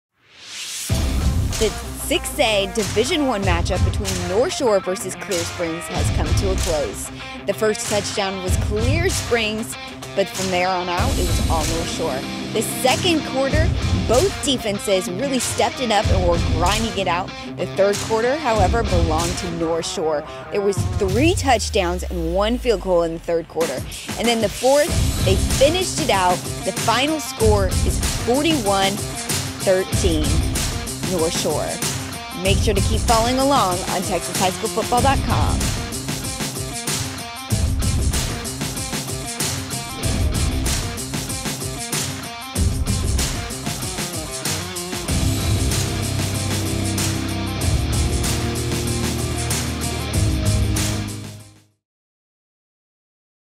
Friday Lights Out - Northshore vs Clear Springs - Wrap Up
Northshore defeated Clear Springs 41 -13 (Northshore) this past weekend.